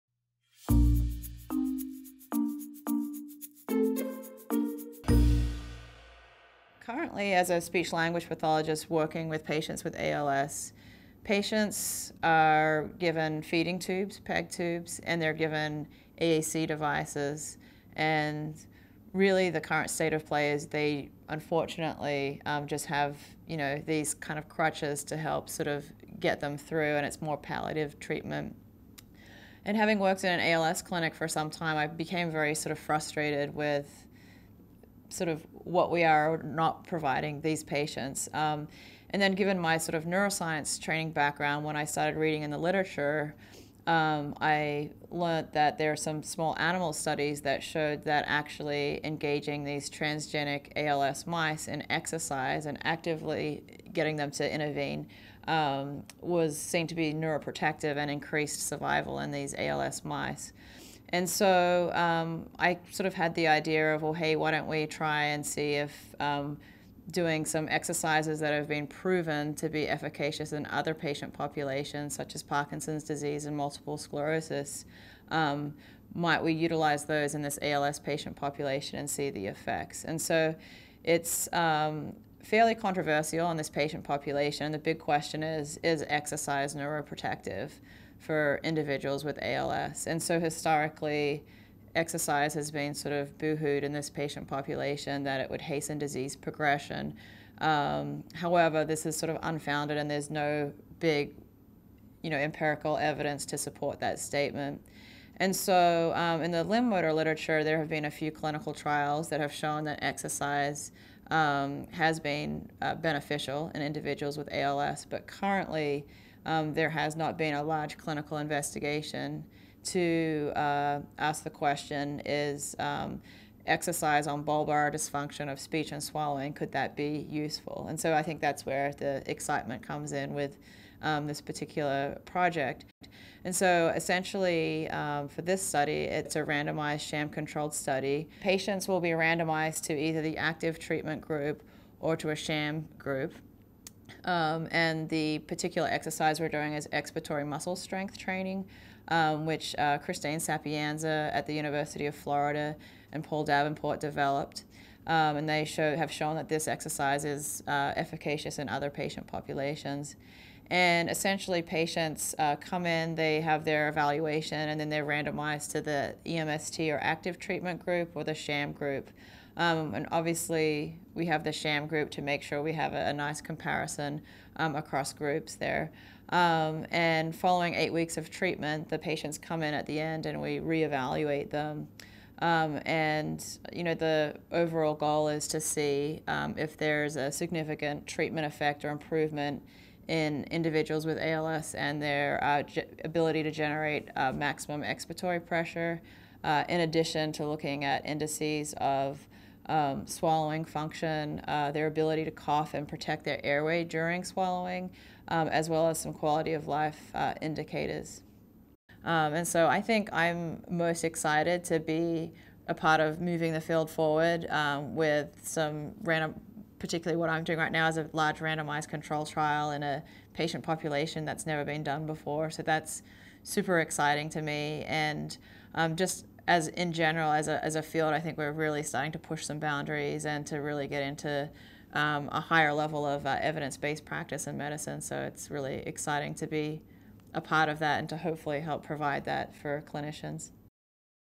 Can Exercise Improve Swallowing for ALS Patients? Behind the Science with Emily Plowman
Video created for the ASHA

An interview with Emily Plowman, University of Florida

In a look "Behind the Science," Emily Plowman talks about her  ongoing investigation and clinical trial studying the role of strength training exercises in helping patients with Amyotrophic Lateral Sclerosis (ALS) to better manage or improve their swallowing.

Read the review article in JSLHR:

Plowman, E. K. (2015). Is There a Role for Exercise in the Management of Bulbar Dysfunction in Amyotrophic Lateral Sclerosis? J Speech Lang Hear Res, 58(4), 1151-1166. doi: 10.1044/2015_JSLHR-S-14-0270